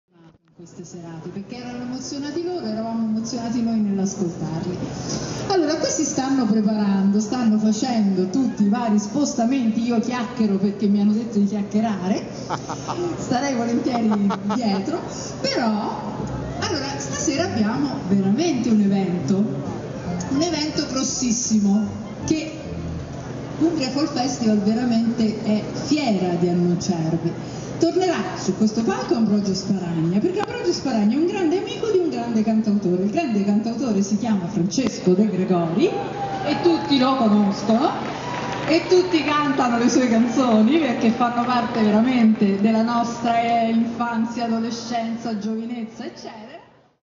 Intrattenendo il pubblico di Umbria Folk Festival 2012
Come intrattenere 3000 persone in una piazza durante il cambio palco ad Umbria folk festival 2012